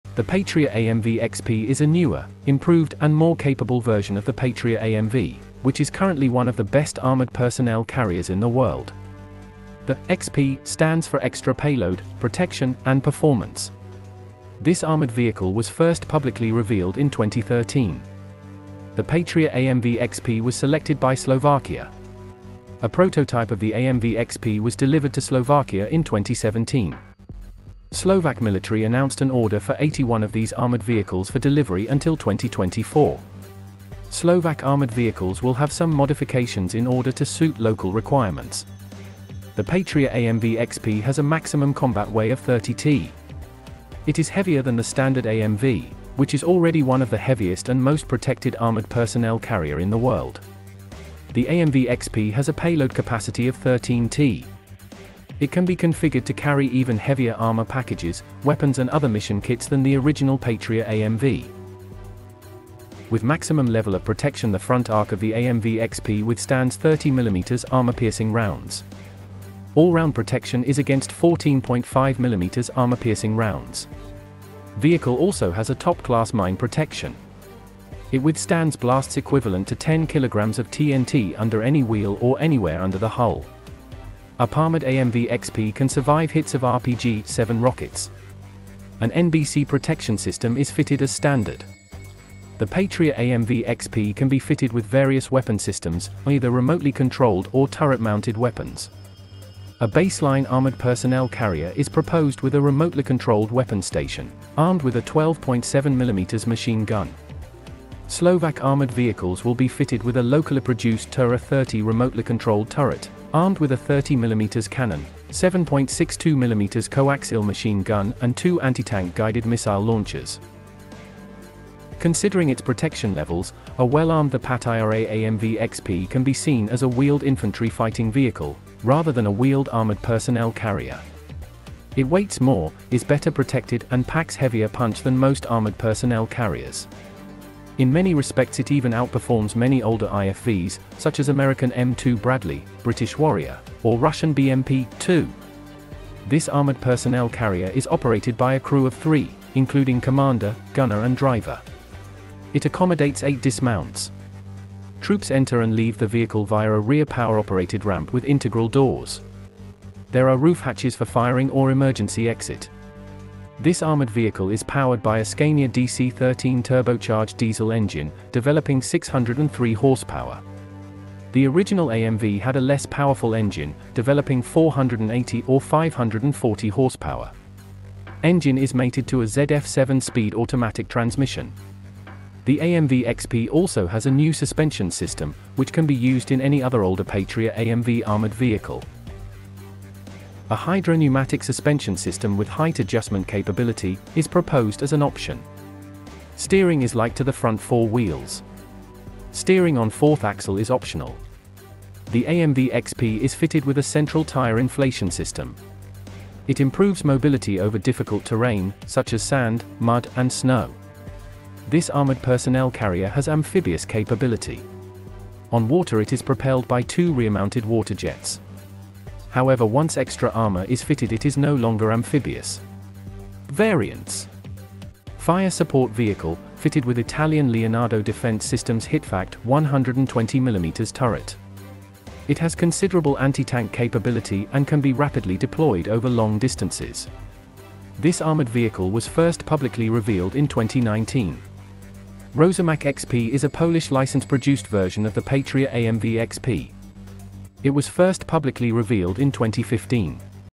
The Patria AMV XP is one of the most protected armored personnel carrier in the world
Write in the comments on which military equipment to make the next review